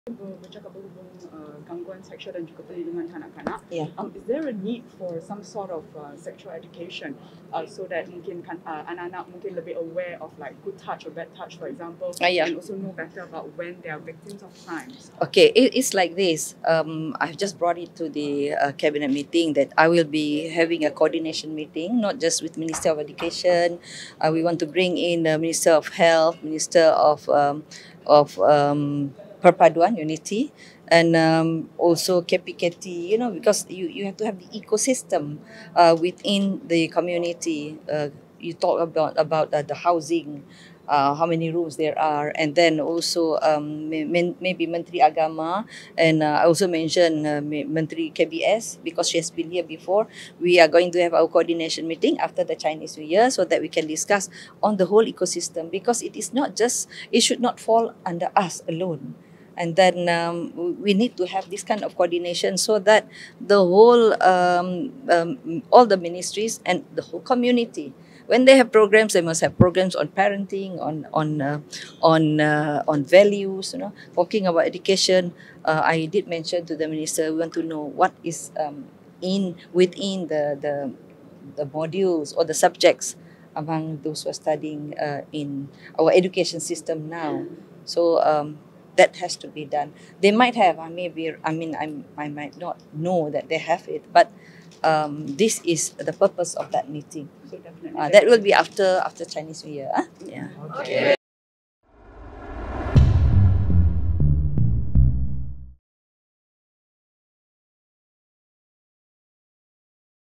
KPWKM akan bincang keperluan pendidikan seksual untuk kanak-kanak
Kementerian Pembangunan Wanita, Keluarga dan Masyarakat (KPWKM) akan menganjurkan mesyuarat penyelarasan dengan kementerian lain yang berkaitan bagi membincangkan keperluan pendidikan seksual untuk kanak-kanak, kata menterinya Nancy Shukri.

Bercakap kepada media hari ini, Nancy berkata perkara itu telah dibangkitkan semasa mesyuarat kabinet untuk menangani isu itu.